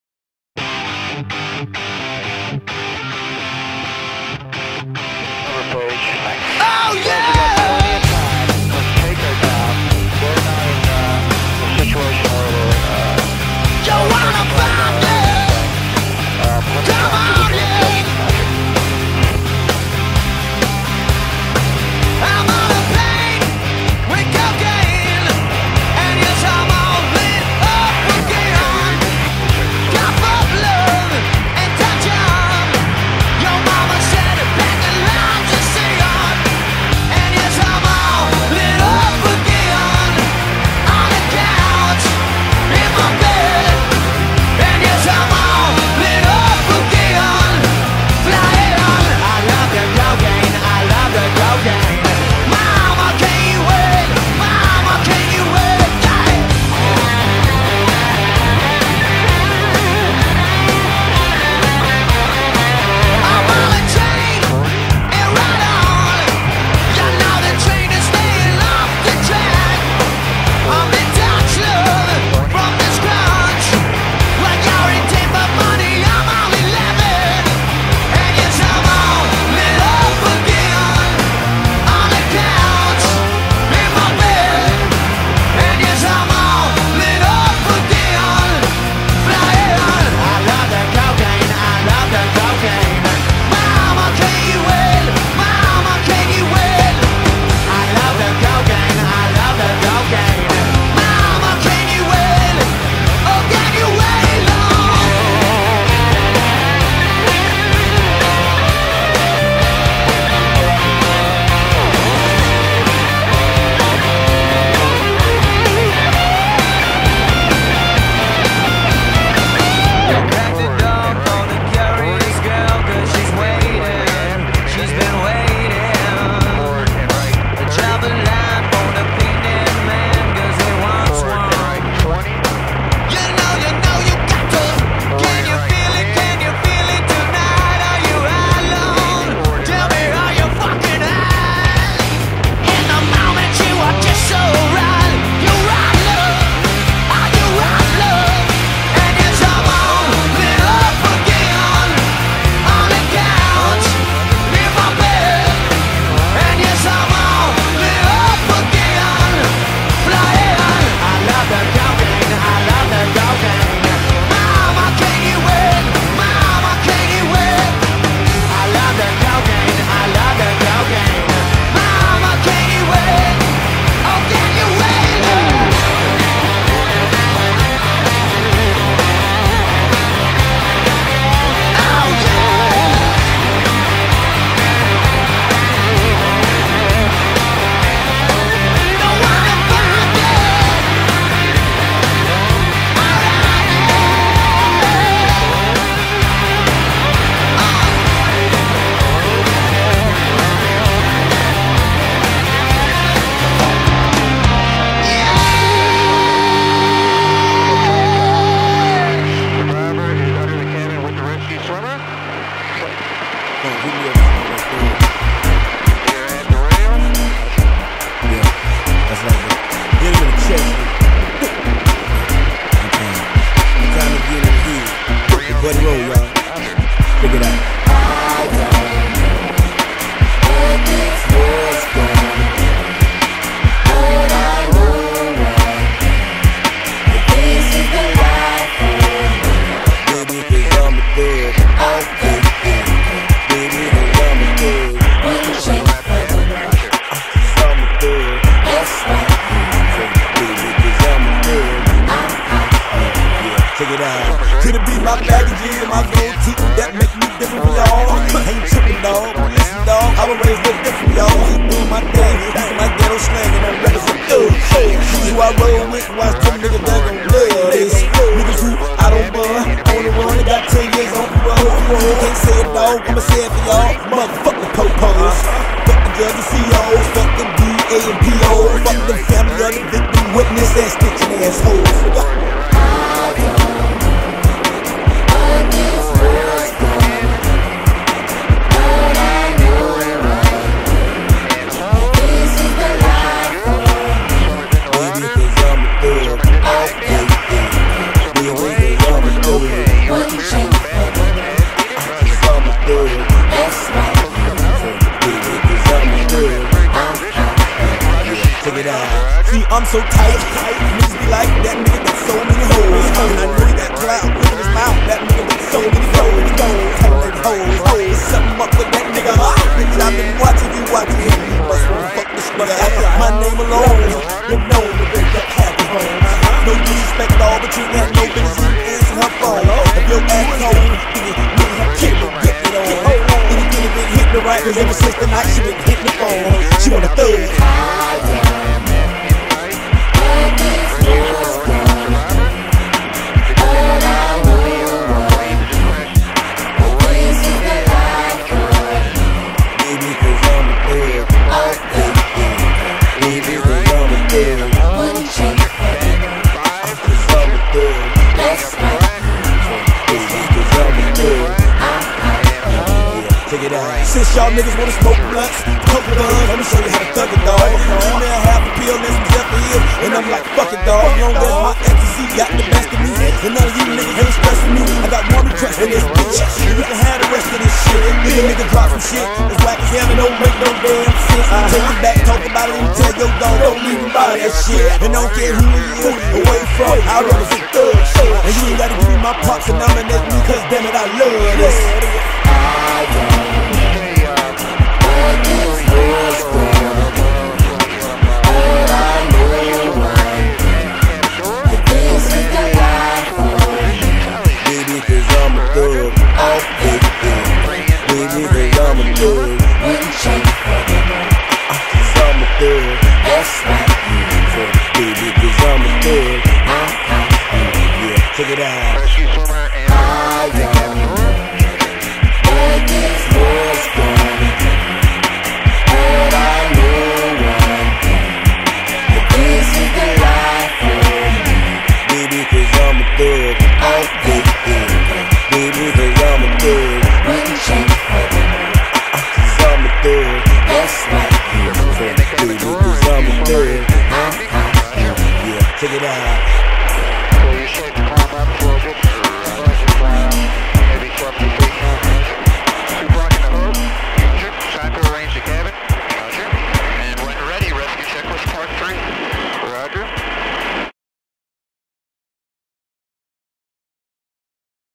USCG rescue hoist
After a sail boat ran aground off the tip of the mitt in lake MI, the USCG is called to pull to people off.